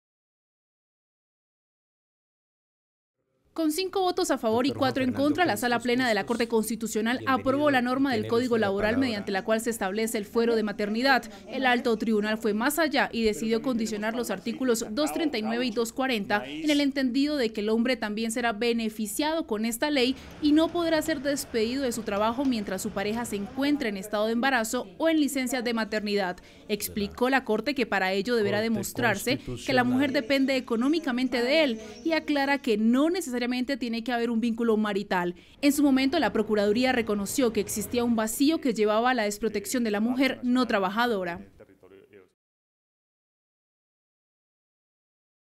'Padres embarazados' no podrán ser despedidos de su trabajo
La Corte aclaró que esto sólo aplicará en casos donde la mujer dependa económicamente de su compañero.

Twitter.com/CMILANOTICIA